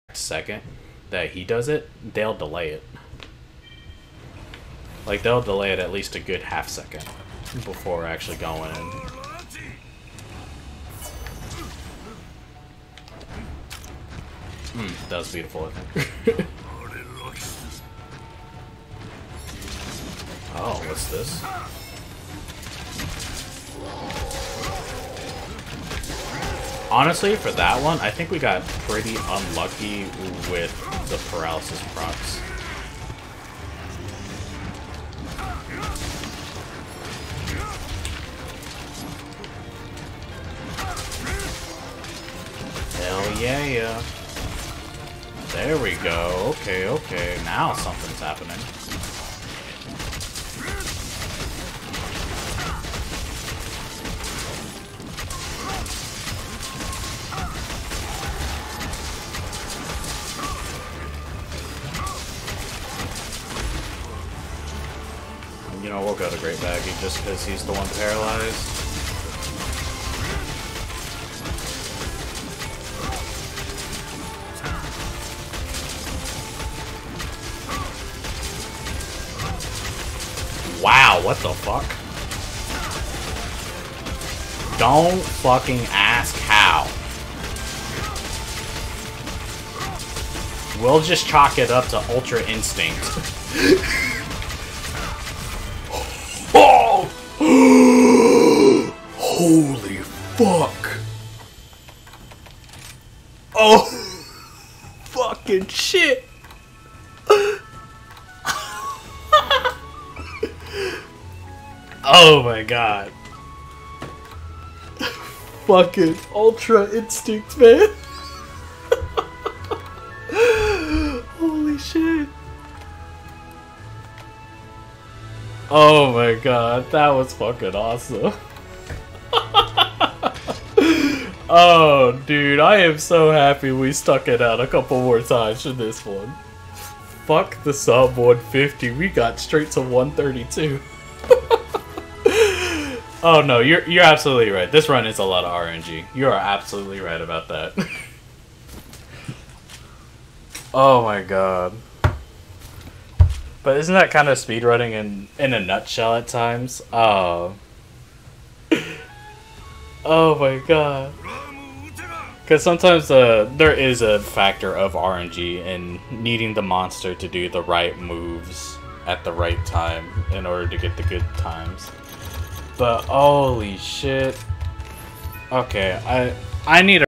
Learning the LBG TA SA 1'32"17 | Twitch VOD
Done during a livestream where I try to finish up running the rest of the HR 4 quests TA style.  The goal was 1'50.  Expectations were surpassed.